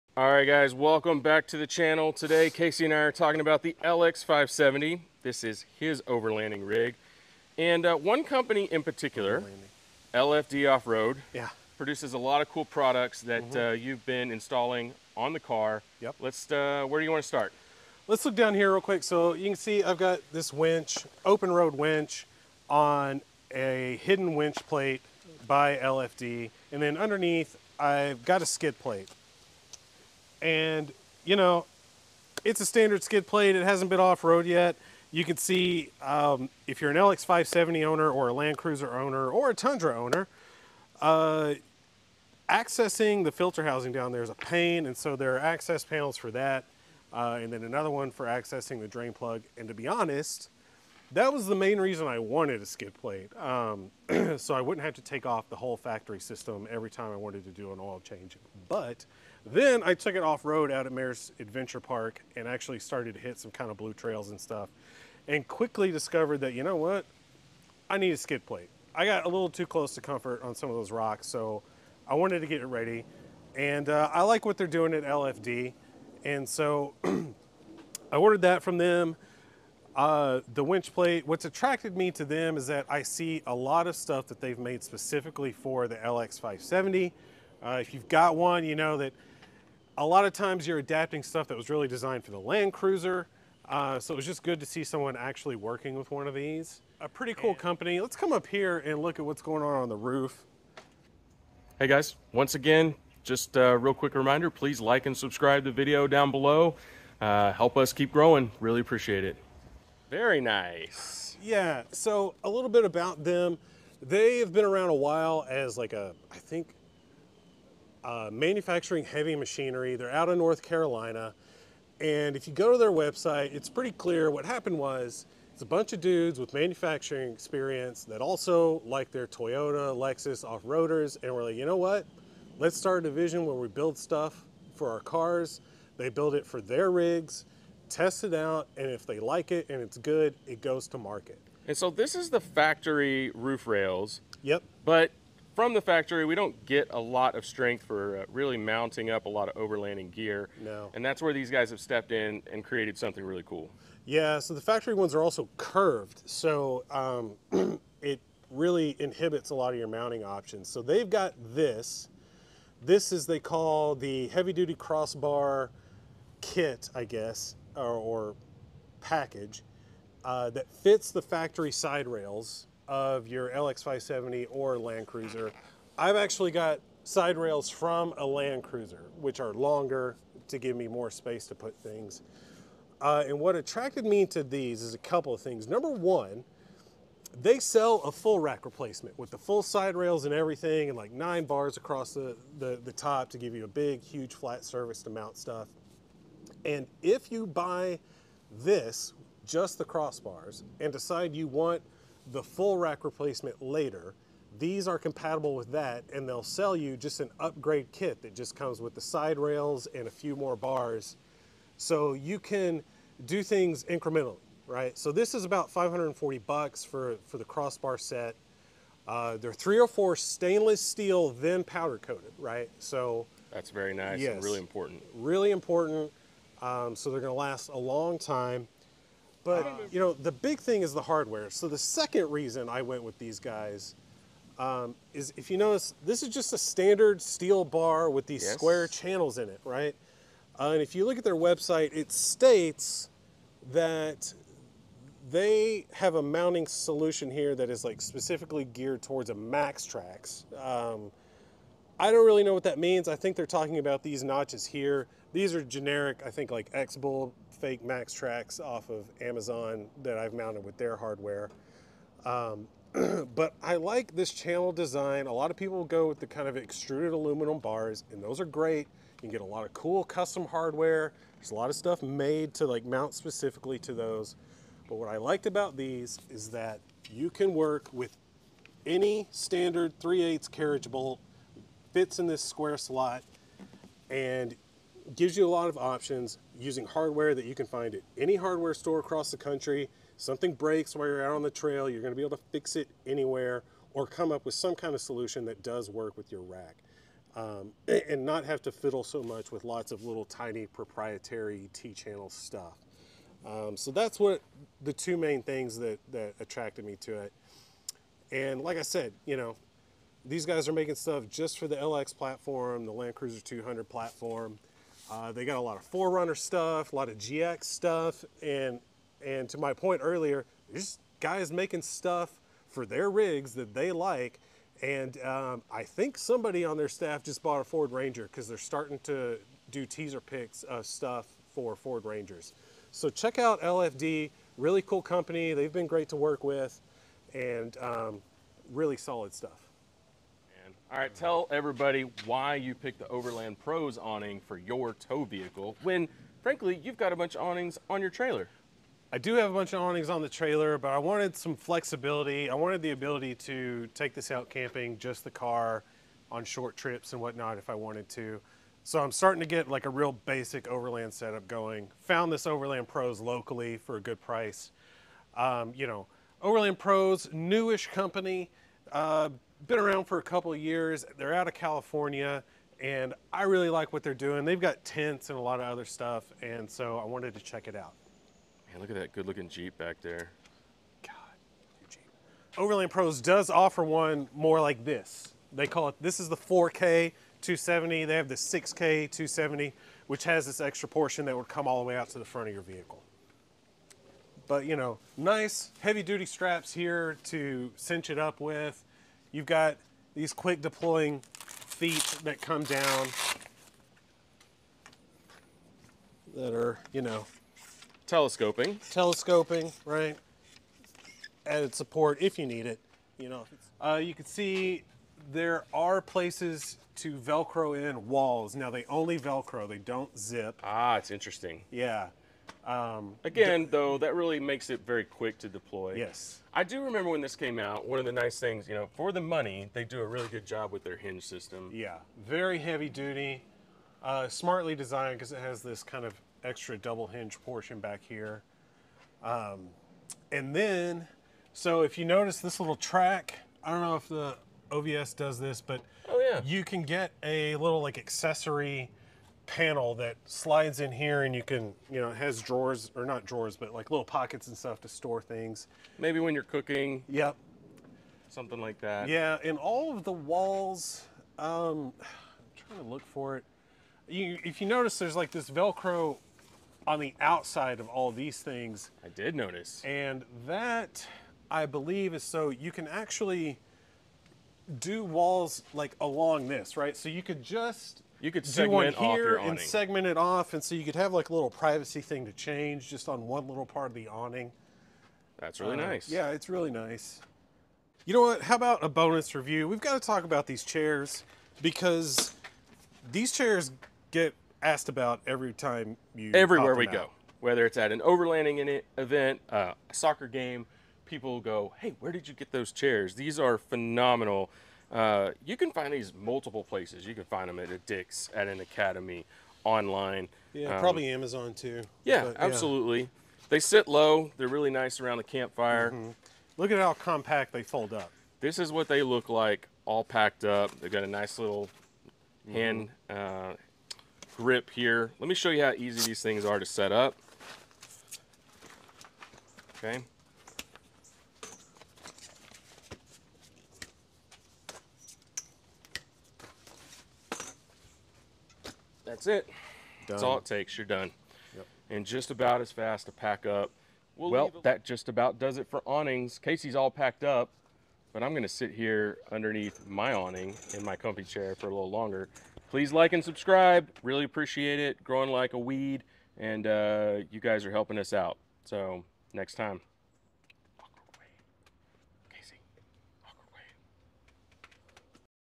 LX570 Overland Prep: LFD Off-Road Skid Plate & Roof Rack, Overland Pros Awning +BONUS review!
Getting the Lexus LX570 ready to go ANYWHERE with purpose built products from LFDoffroad and @overlandpros7572